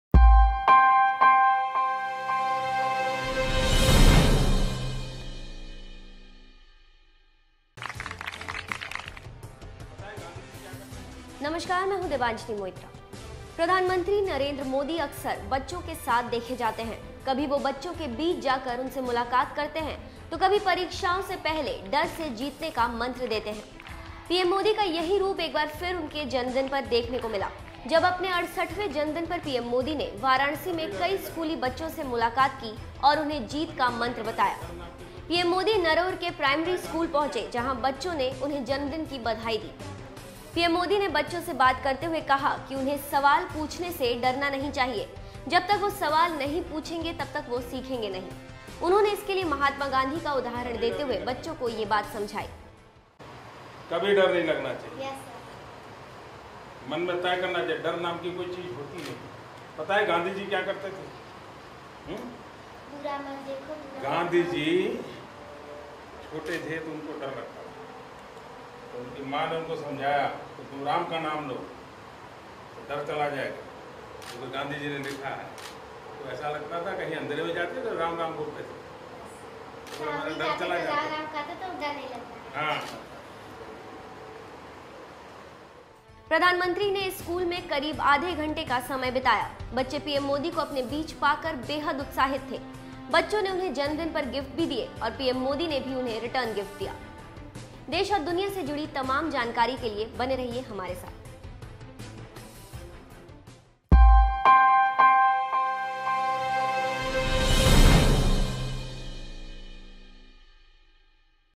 Modi ने Birthday स्कूली बच्चों के साथ बिताया समय, सिखाया जीत का फॉर्मूला
प्रधानमंत्री नरेन्द्र मोदी अपने 68वें जन्मदिन पर संसदीय क्षेत्र वाराणसी में थे...इस दौरान उन्होंने यहां के नरउर स्कूल के बच्चों से भी मुलाकात की...पीएम मोदी ने बच्चों के कई सवालों को खुलकर जवाब दिया...  Watch this video to know more.